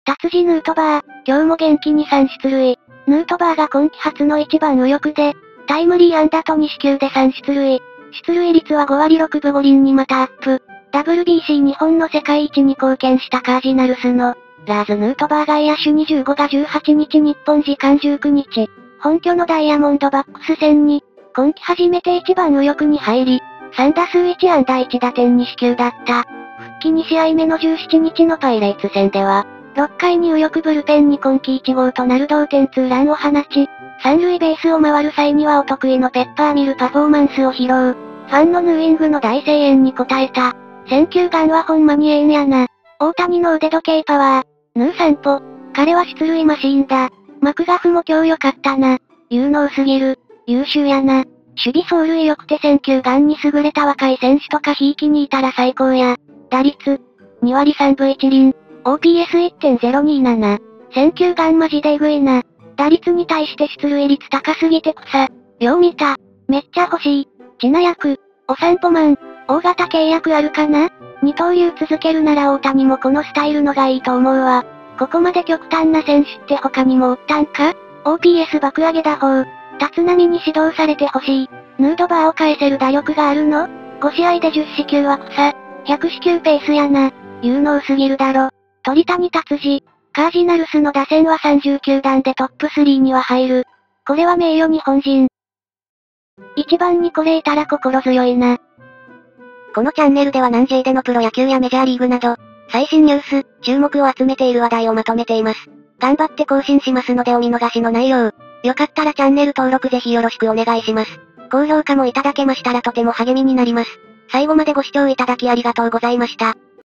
135 【タツジ】ヌートバー、今日も元気に3出塁【なんJ反応】
ヌートバーが今季初の「１番・右翼」でタイムリー安打と２四球で３出塁　出塁率は・５６５にまたアップ！！
なんJでの反応をお楽しみください！！